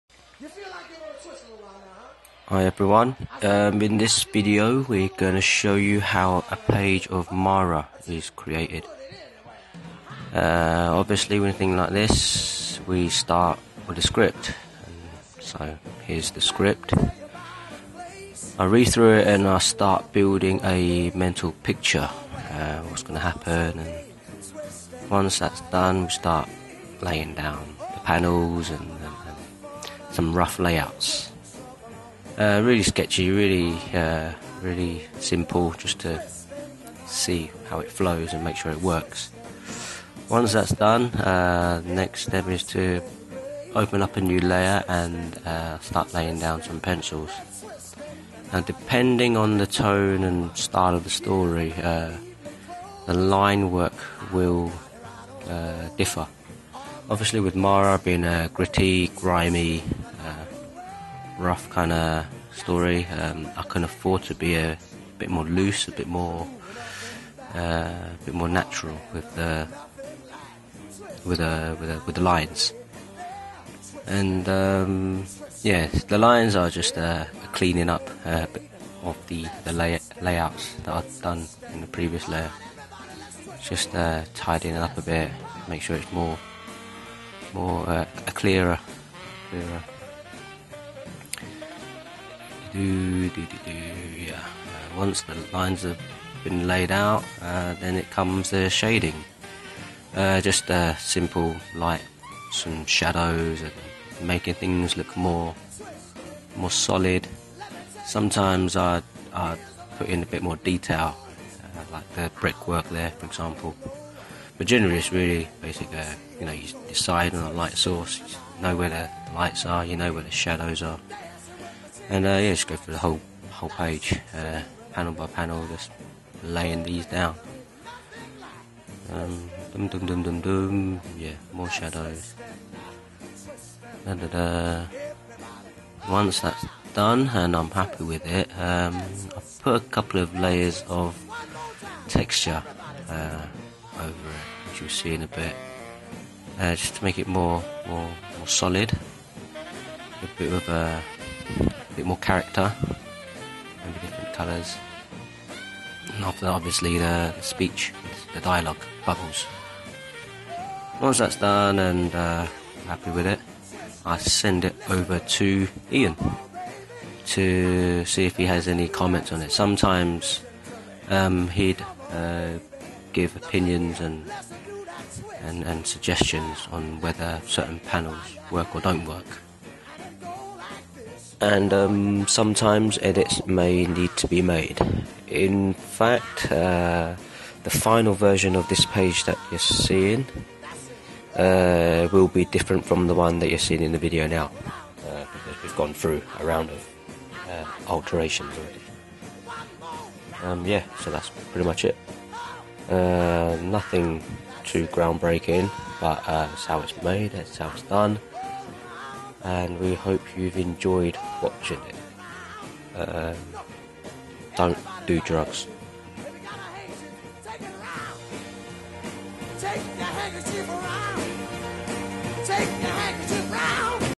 Daniel Tang draws Mara
Daniel Tang, co author of the serialised graphic novel 'Mara' talks through drawing a page from Mara Part 2. Watch him build the page from beginning to end.